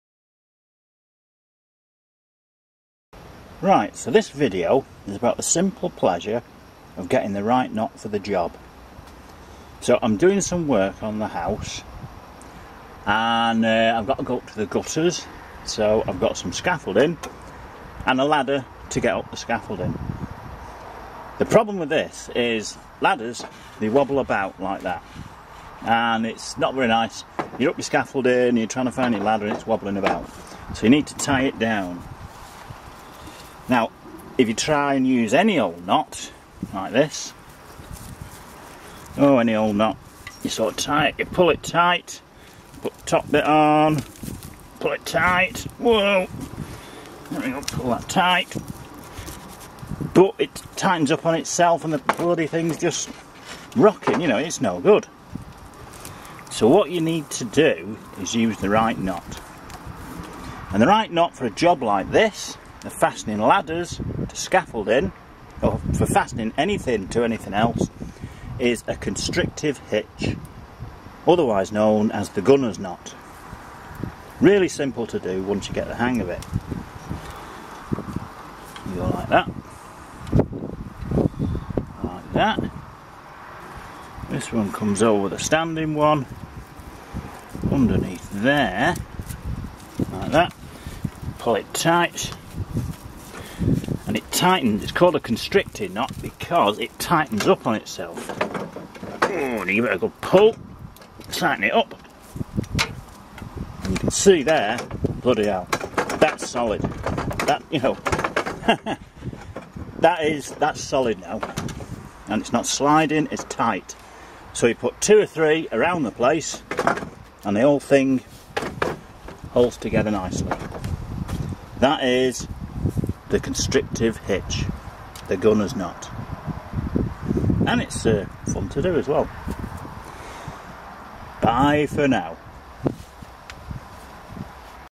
Simple Pleasures 15 - The Gunners Knot
Getting the right knot that holds and ties stuff tightly is very satisfying.
When things have to be held tightly together, such as ladders to scaffolding,  the Gunners Knot or Constrictive Hitch is one of the best things to use.